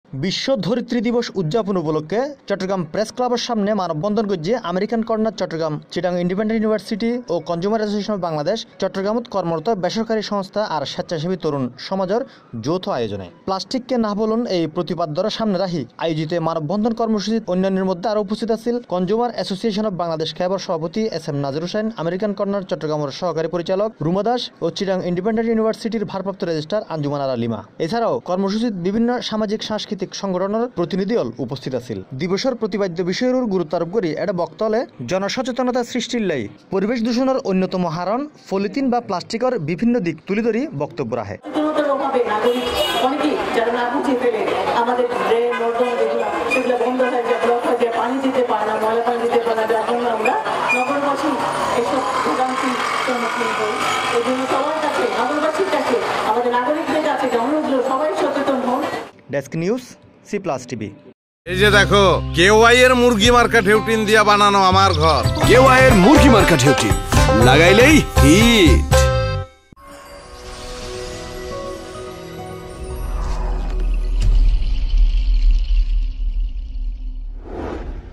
বিশ্ব ধরিত্রি দিবস উদযাপন উপলক্ষে মানব বন্ধন কর্মসুচি অনুষ্ঠিত হয়েছে
বিশ্ব ধরিত্রি দিবস উদযাপন উপলক্ষে মানব বন্ধন কর্মসুচি অনুষ্ঠিত হয়েছে। প্লাসটিককে না বলুন এ শ্লোগানে আজ বিকালে চট্টগ্রাম প্রেস ক্লাবের সামনে এই মানববন্ধন পালন করা হয়েছে।